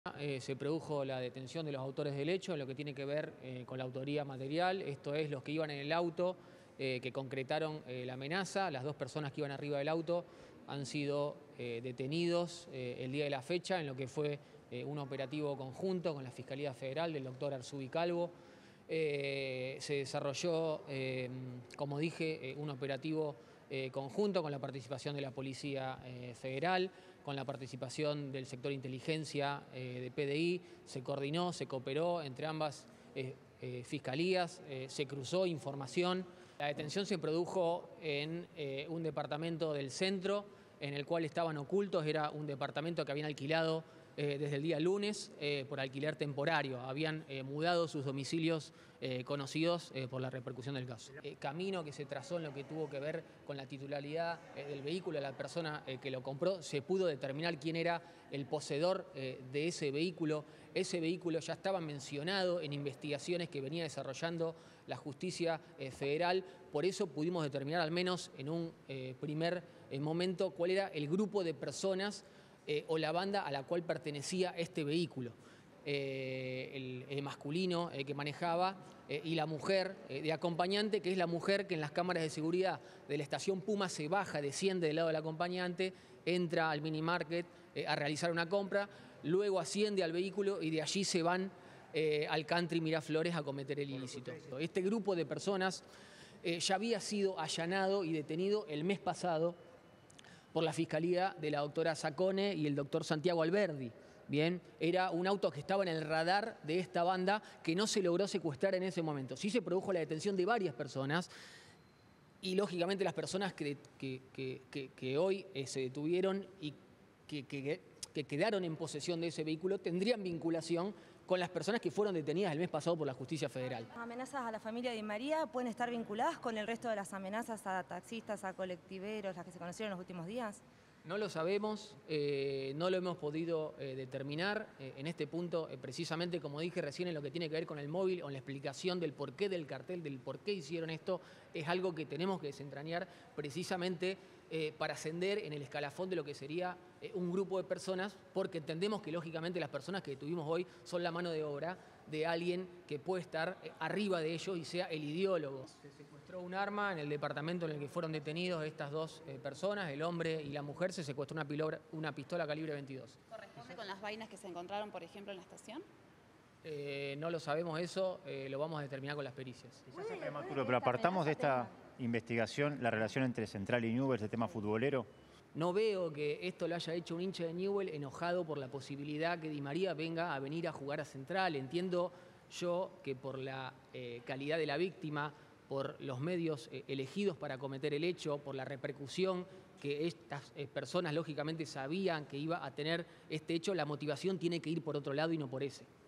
Caso Di María: detienen a los autores materiales de la amenaza
Luego de un trabajo de investigación minucioso, en un operativo conjunto entre la policía provincial y la policía federal, arrestaron a una pareja acusada de ser autora material de la amenaza contra la familia de Ángel Di María.